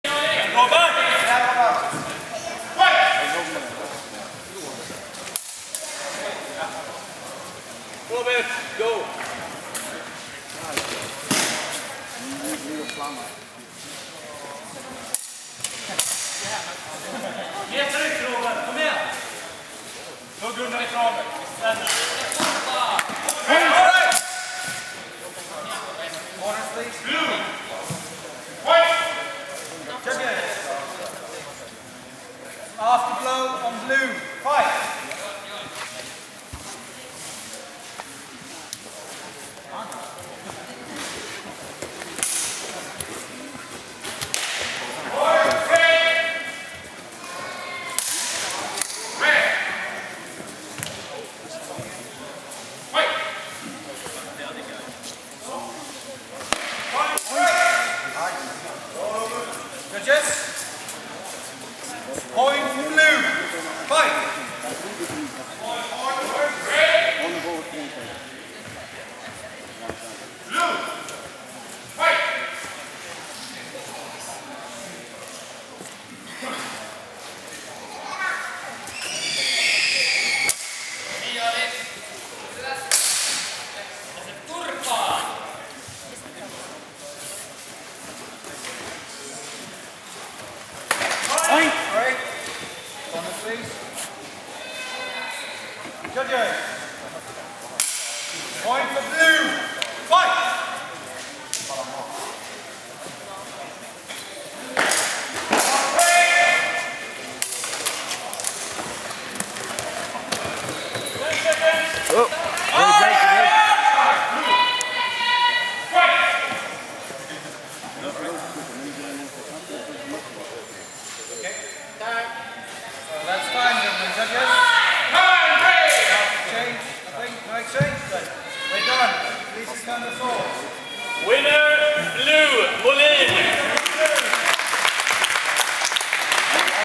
Robert Molin (blue) vs Joeli Takala (red) - longsword - swordfish 2011
Swordfish 2011 longsword tournament